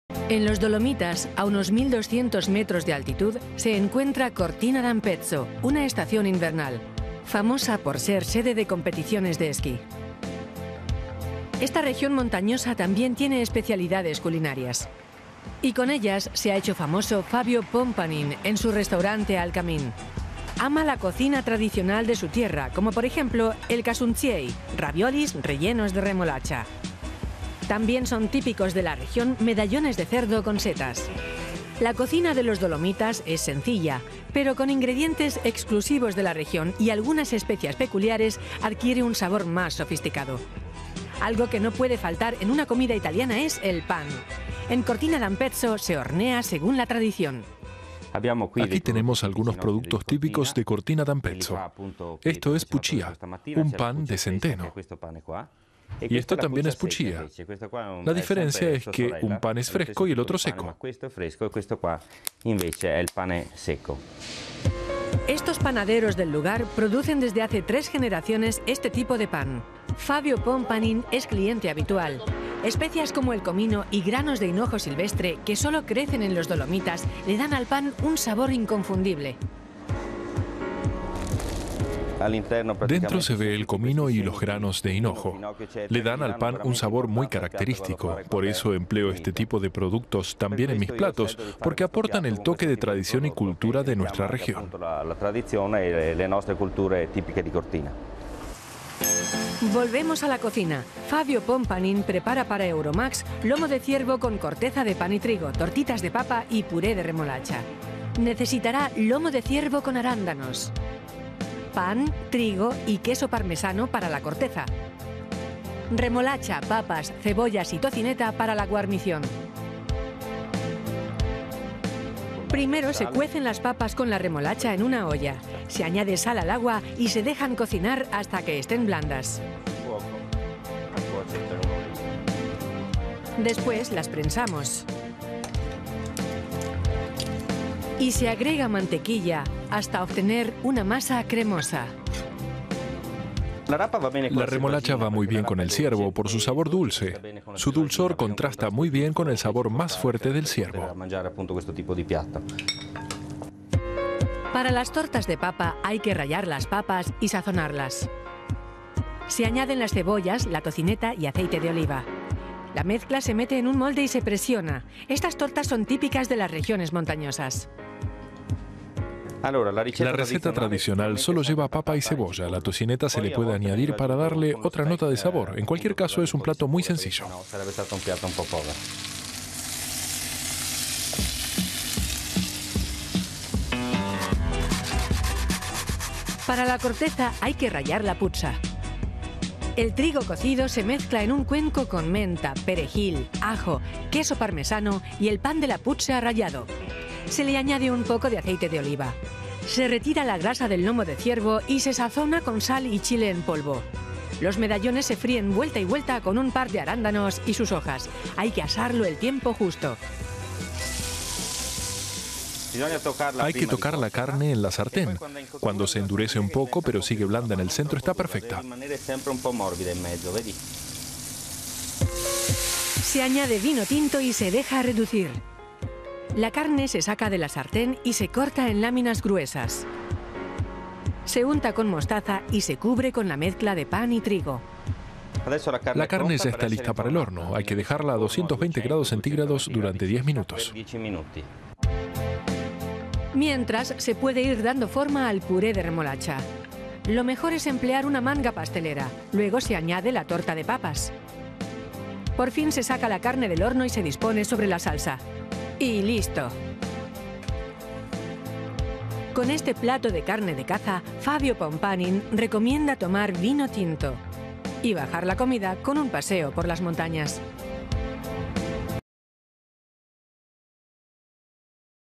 Lomo de ciervo con corteza de pan | Euromaxx
Cortina d’Ampezzo, en el norte de Italia, es famosa por sus diversos tipos de pan. Una variedad es "La puccia”, a base de centeno e hinojo. El chef Fabio Pompanin cocinó para Euromaxx un lomo de ciervo con corteza de puccia.